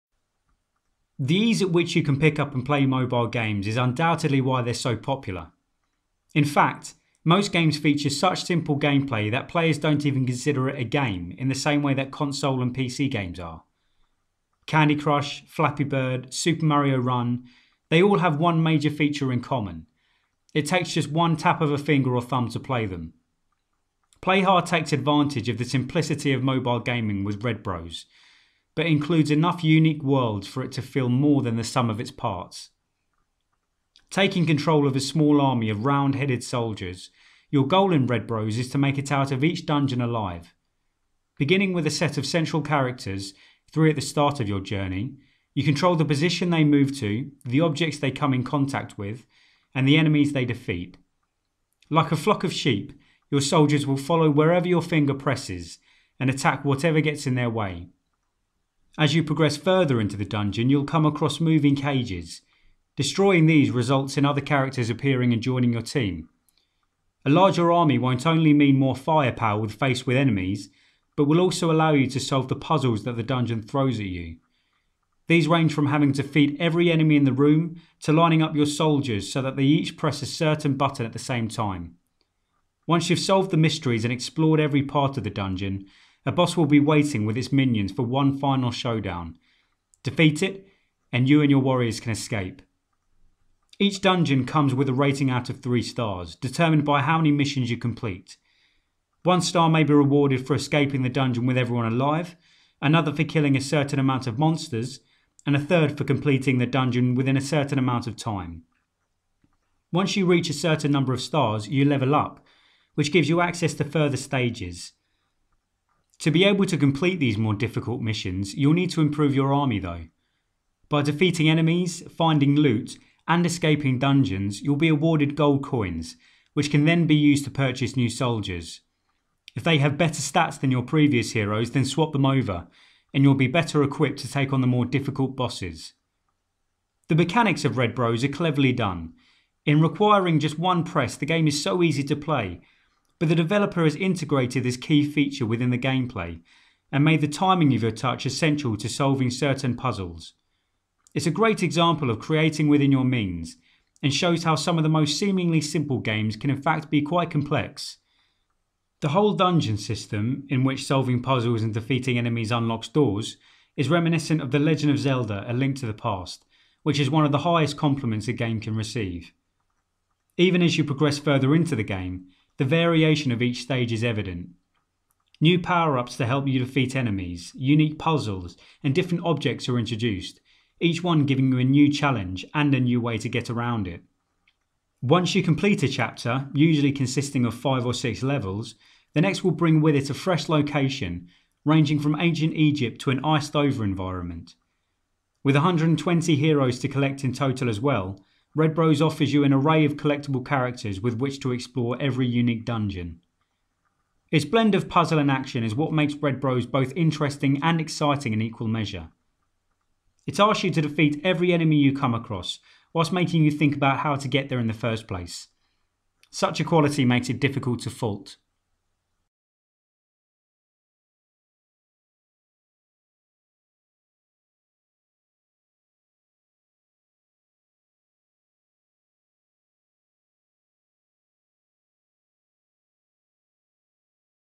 Red Bros Review | AndroidApps.com
Its blend of puzzle and action is what makes Red Bros both interesting and exciting in equal measure.